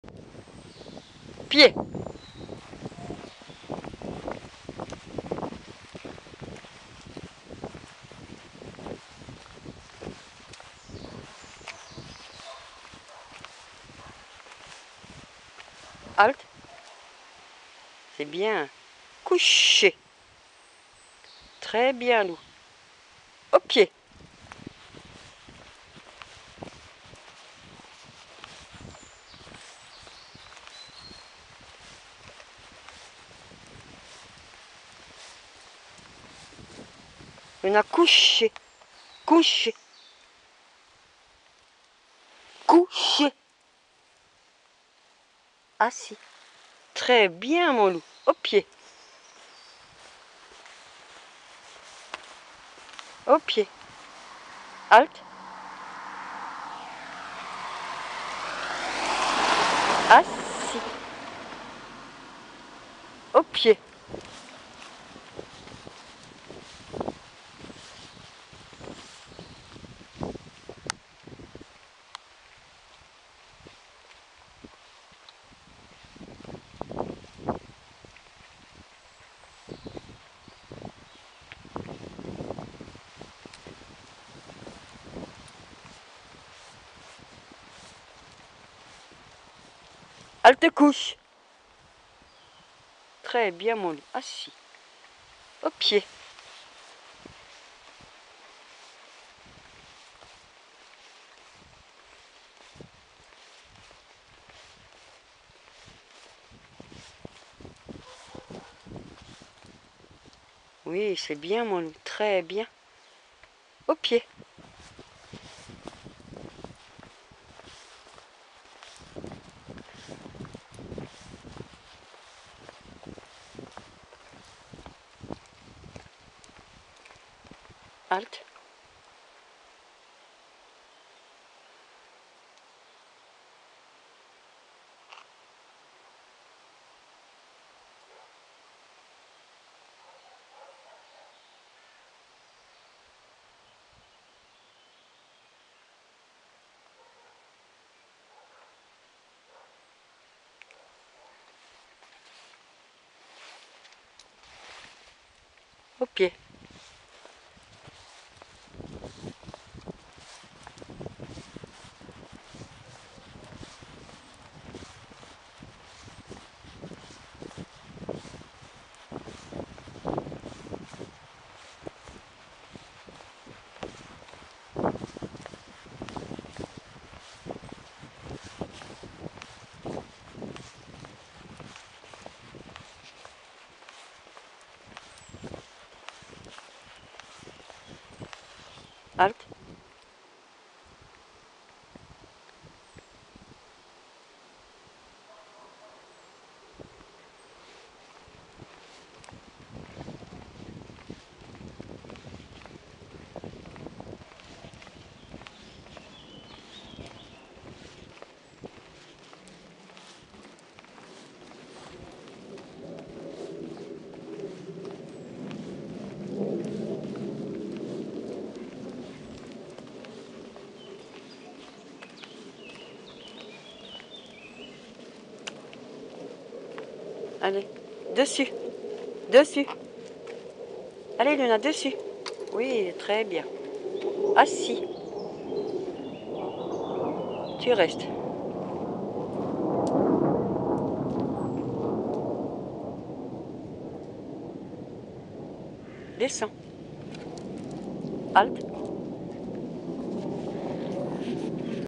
Marche aux pieds en Laisse pour Louna
Petit entrainement pour Louna ce matin ,sur le chemin du retour vers le parking .